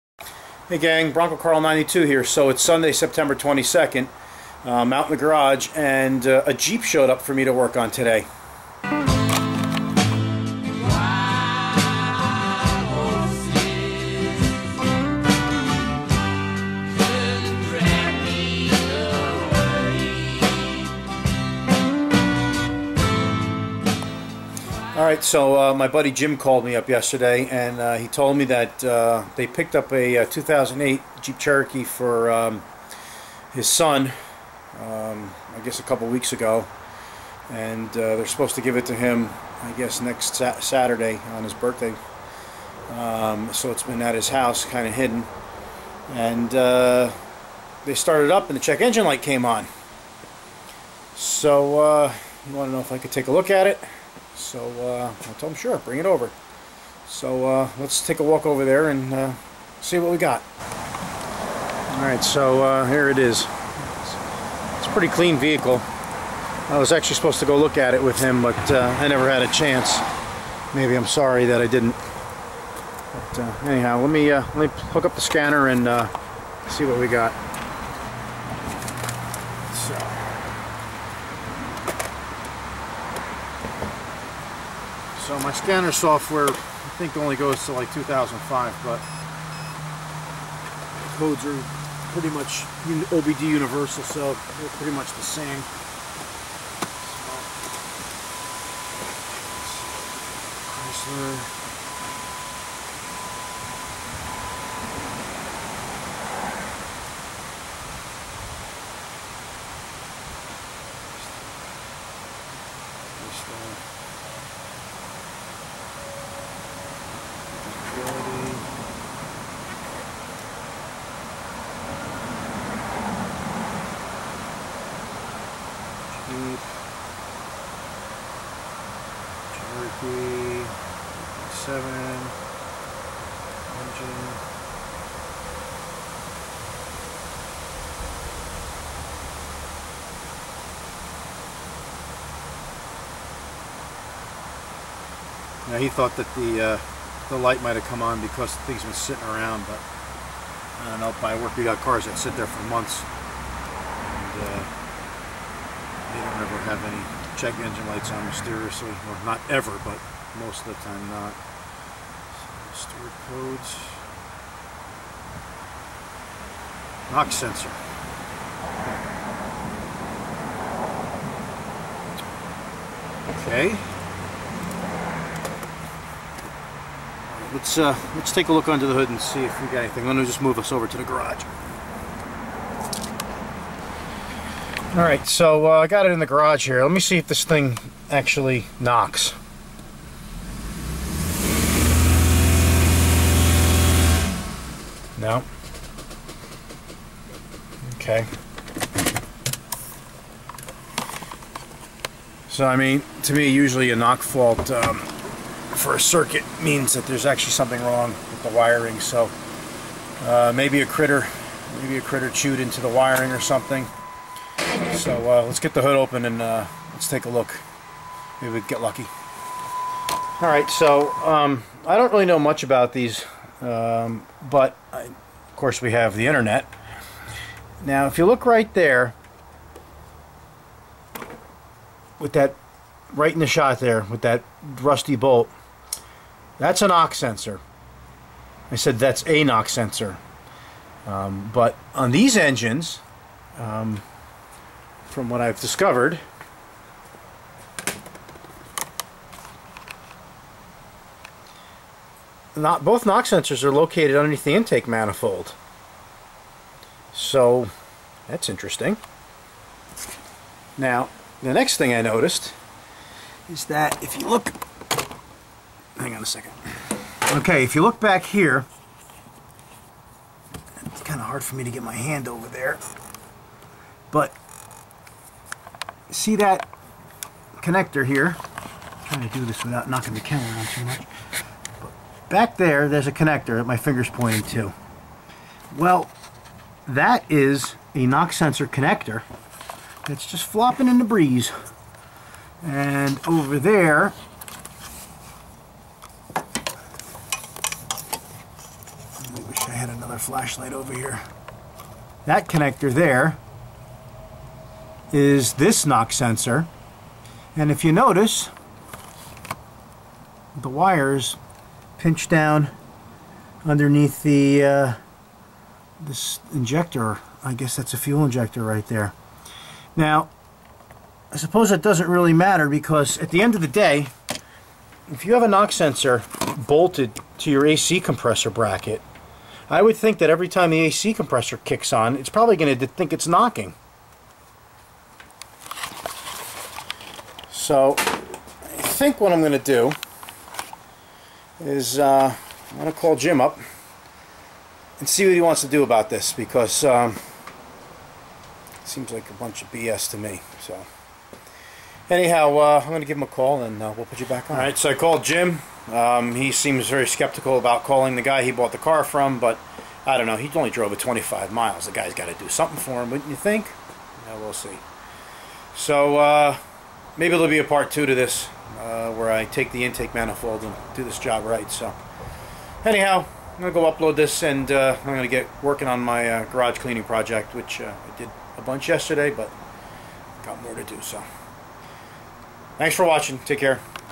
9-22-13 2008 Jeep Grand Cherokee 3.7 code P0325 Part 1?
Our friends picked up this used car for their sons 17th birthday. It only got 25 miles before the check engine light came on. Look at what I found, more nice mechanic work...

Part 2 coming???